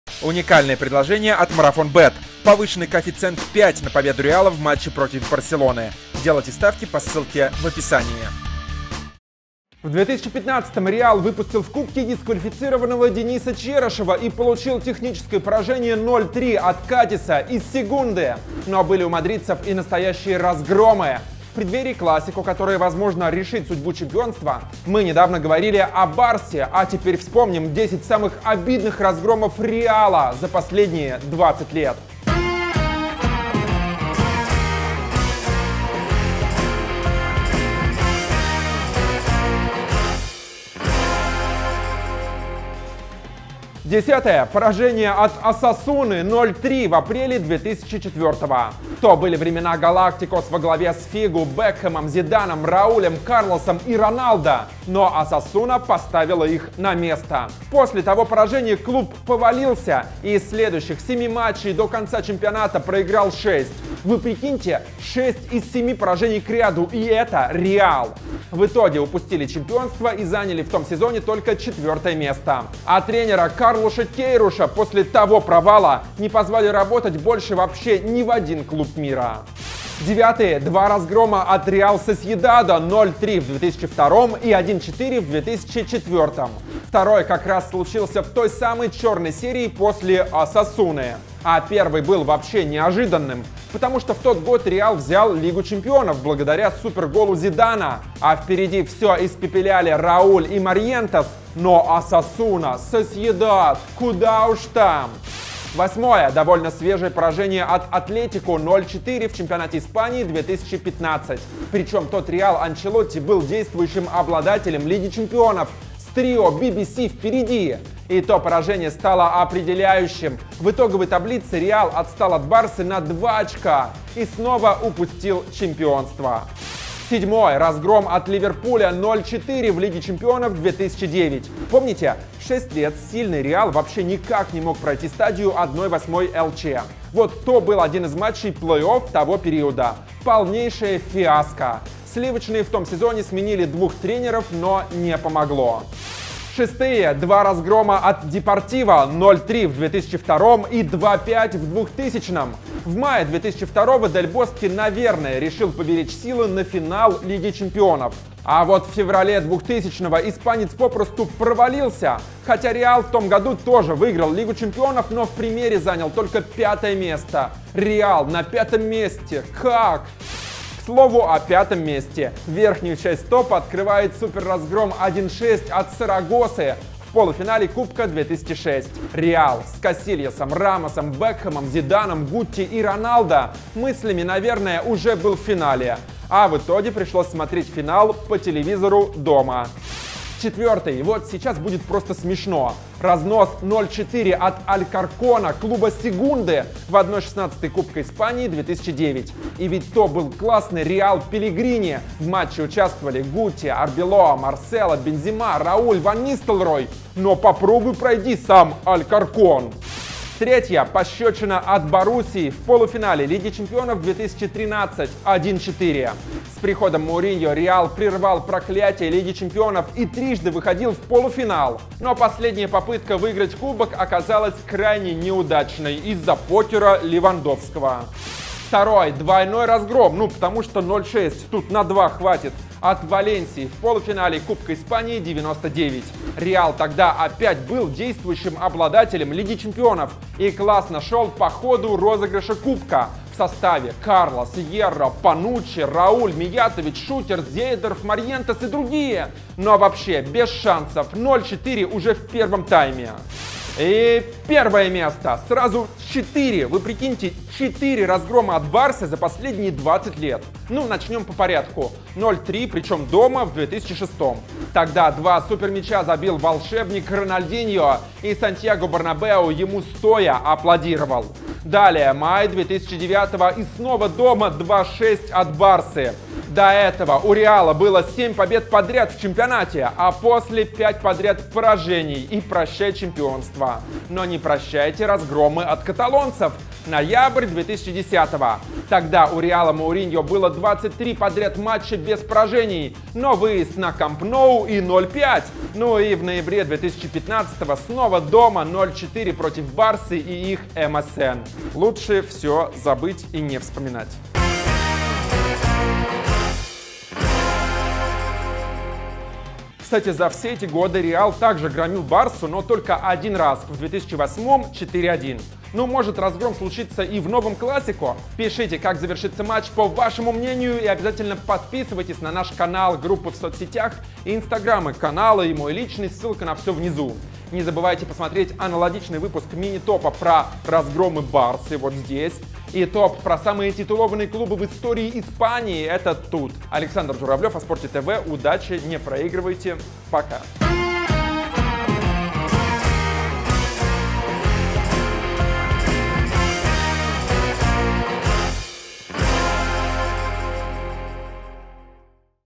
Мини-ТОП-10 разгромов "Реала"
Накануне ЭЛЬ-КЛАСИКО вспоминаем то, что внушает "Барселоне" надежду!